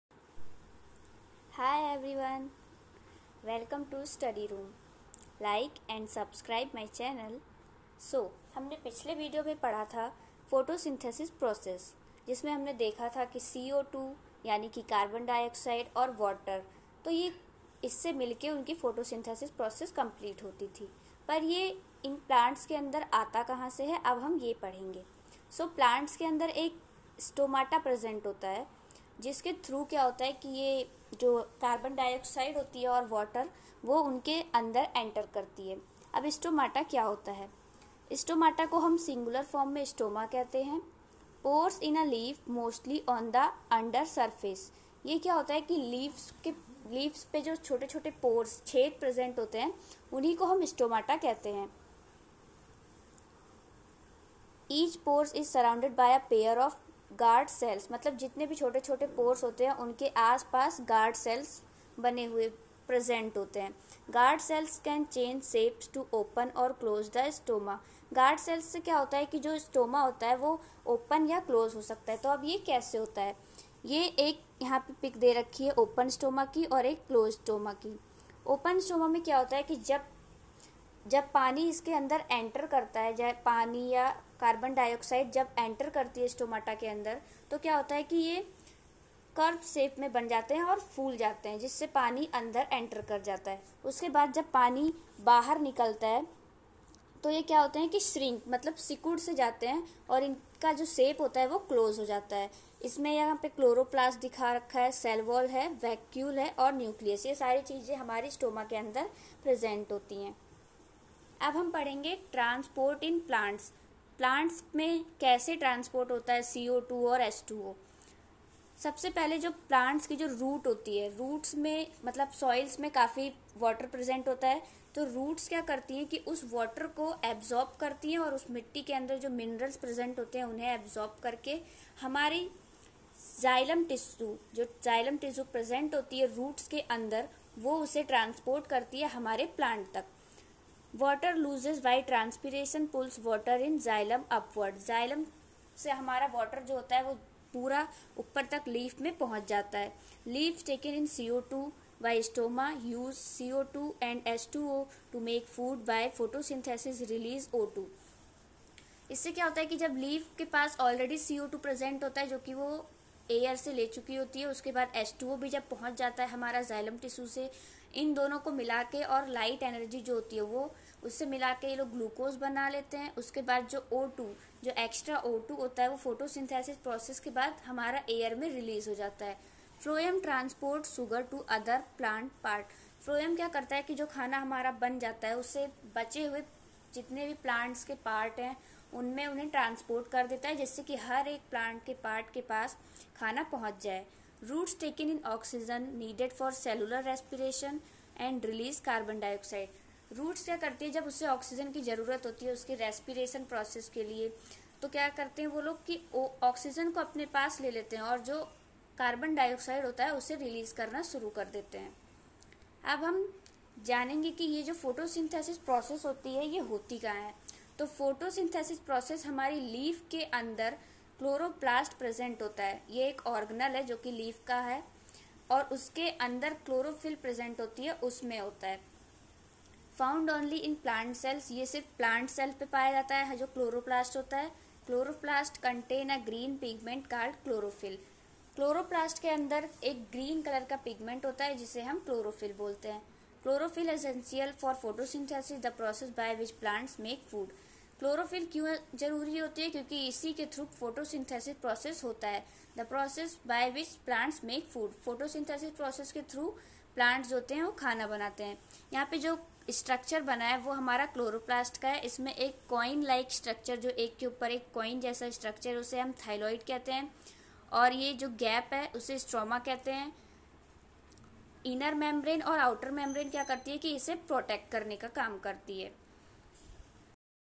Life process part 3 by shikha
Detailed explanation of how plant obtain carbon dioxide and water. You all also comment your doubt on comment box